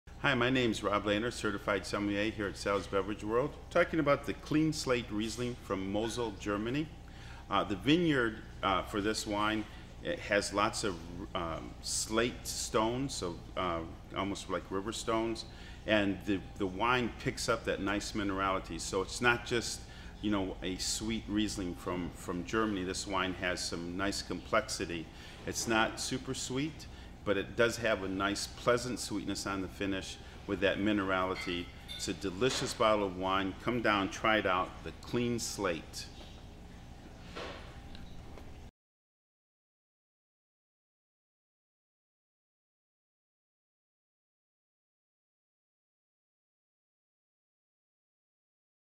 Clean Slate Riesling
Fresh, balanced taste is achieved by carefully selecting grapes from vineyards throughout Mosel. The steep blue slate slopes of the Lower Mosel give the wine its minerality while the Middle Mosel provides a hint of spice to complement the characteristic fresh peach flavors of the Upper Mosel. Ripe peach flavors balance ripe acidity, hints of lime and characteristic mineral notes.